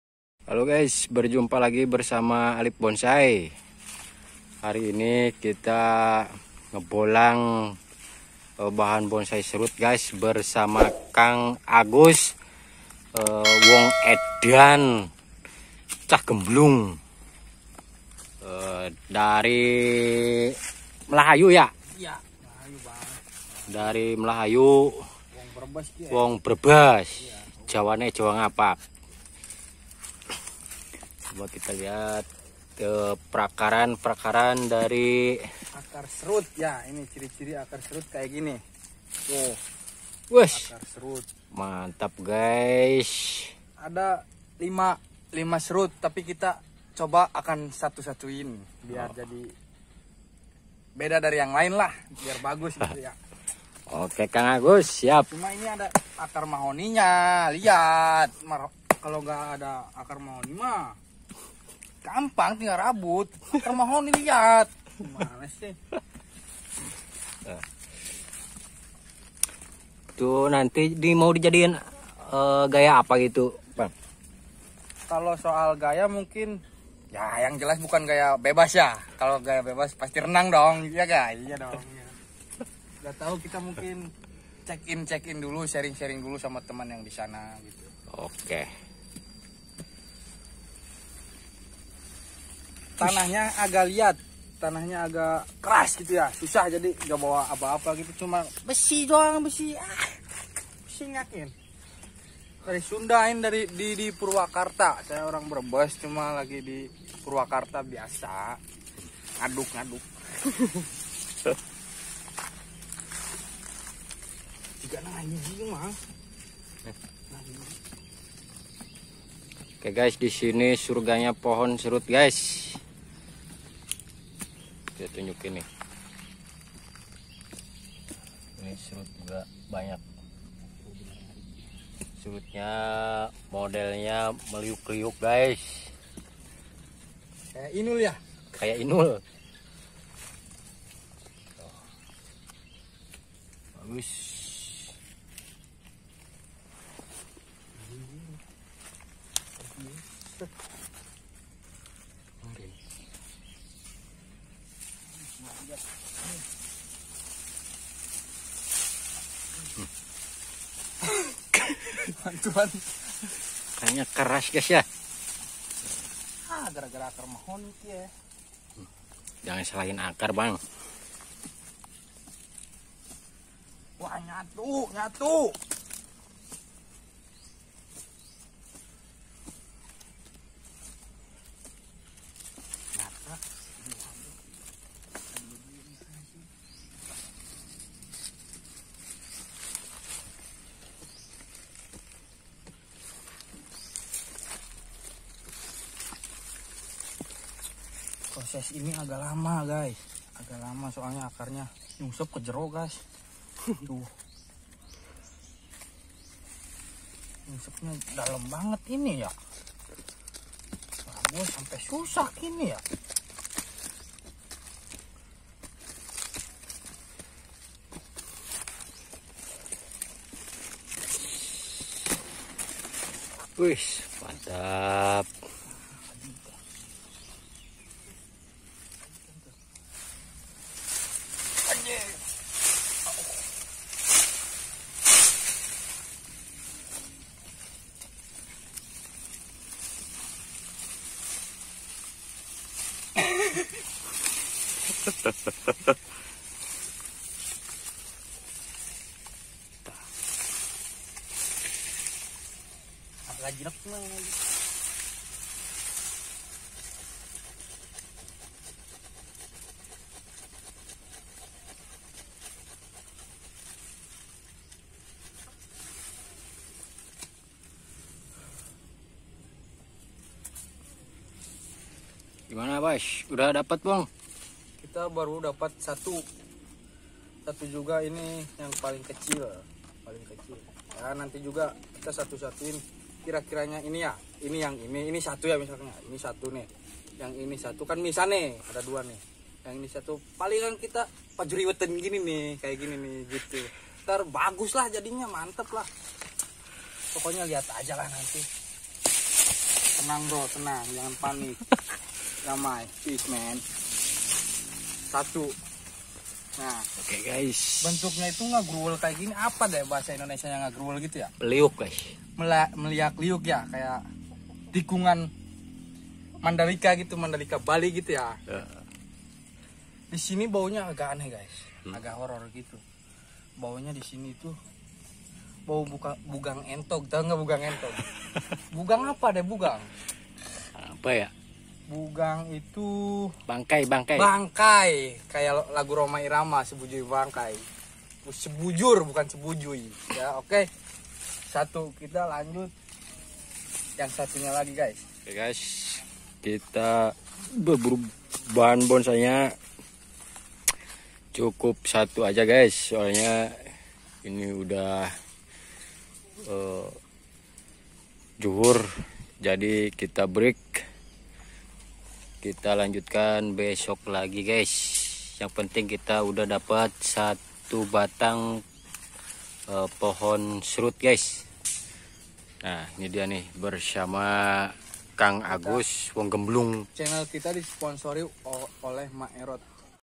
BERBURU BAHAN BONSAI SERUT!!SUPER ISTIMEWA !!SURGA YA POHON SERUT
kali ini saya berburu bahan bonsai serut di pinggiran pemukiman warga dan gak di sangka banyak serut yg belom terjamah sama Orang. auto langsung sikat